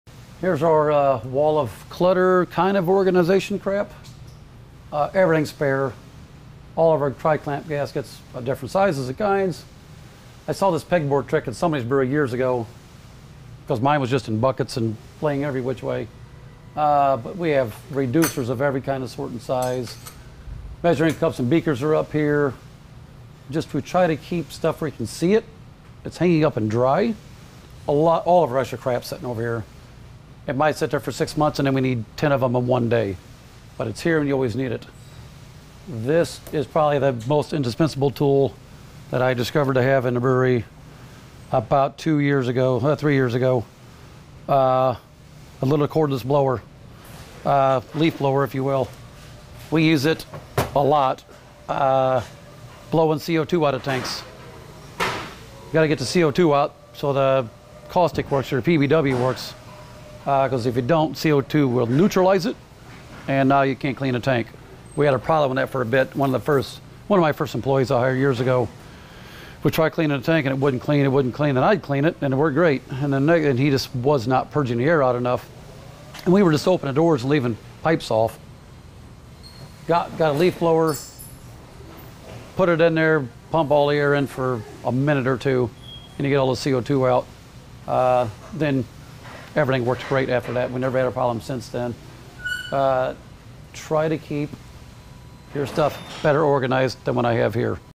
Keep Your Bits and Bobs in Order
Nobody wants to turn their brewhouse upside-down hunting for a measly clamp or gasket. In this video tip, 2nd Shift founder-brewer-handyman Steve Crider shows how they keep things sorted—and reveals a secret weapon for cleaning kegs.

Check out the full course on 'How to Fix Sh*t in Your Brewery with 2nd Shift Brewing'